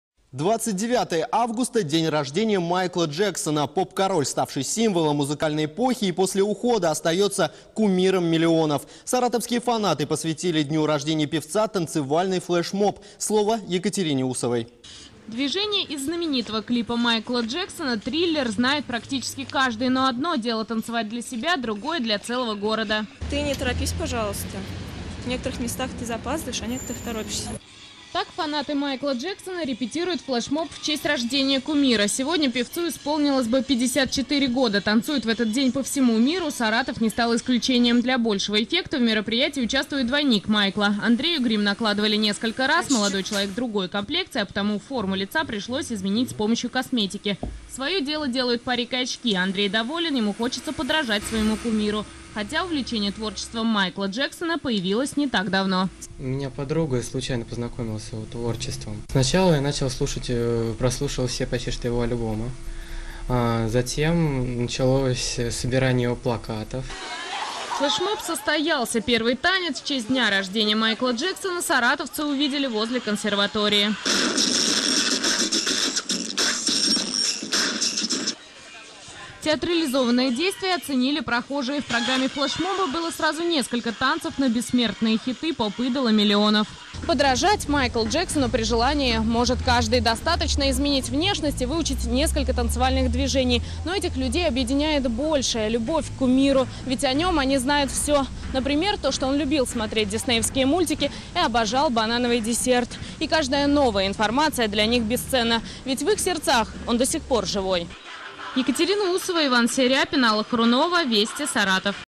День рождения поп-короля отметили в Саратове
Саратовские фанаты посвятили Дню рожденья певца танцевальный флэш-моб.
Источник:  gtrk-saratov.ru